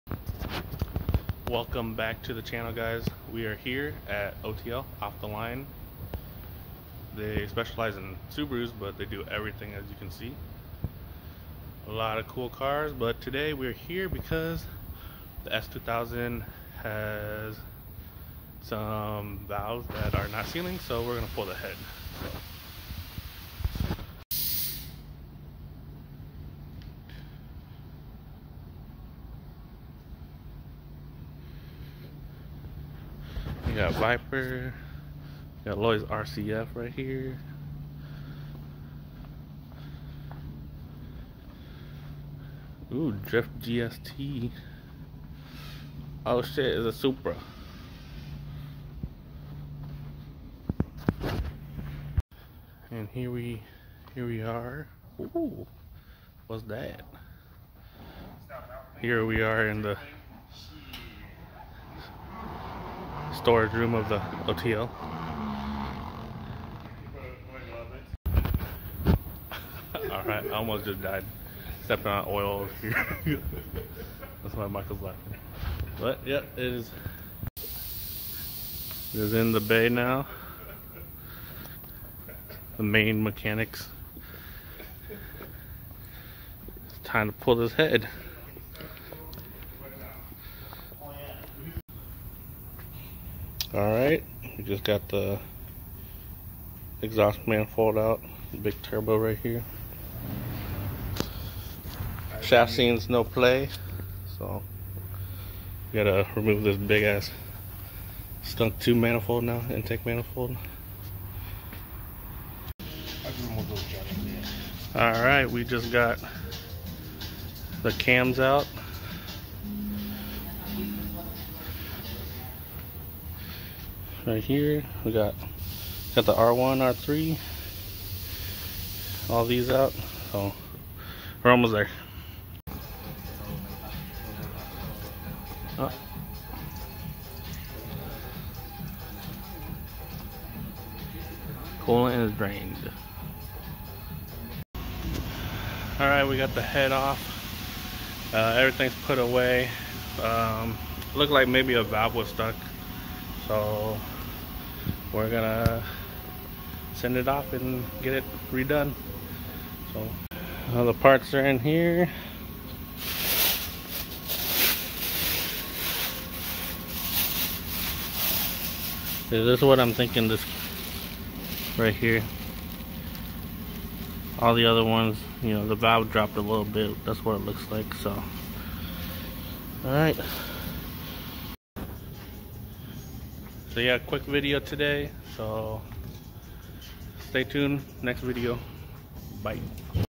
Turbo CR S2000 lost compression!
while racing a Ford Raptor the CR started acting funny, found it there was no compression in cylinder 3!

so we pulled the head to get rebuilt with new valves, springs and retainers.